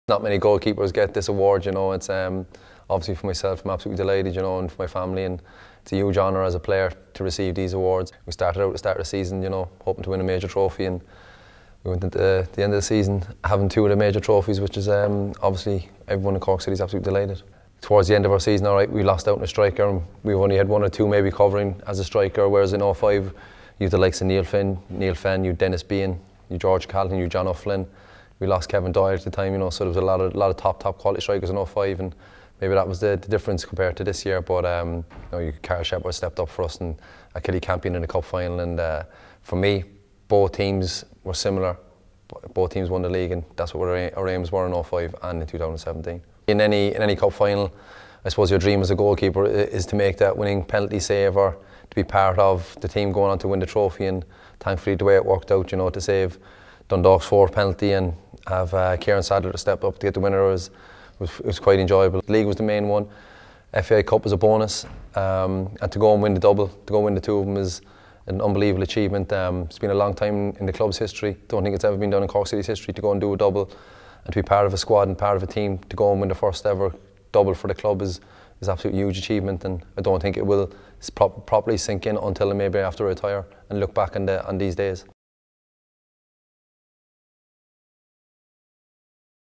SSE Airtricity SWAI Player of the Month Award for November
Congratulations to Mark McNulty of Cork City FC.

SSE Airtricity is proud to power Irish football.

To find out more about 100% Green Energy and the latest offers from SSE Airtricity, simply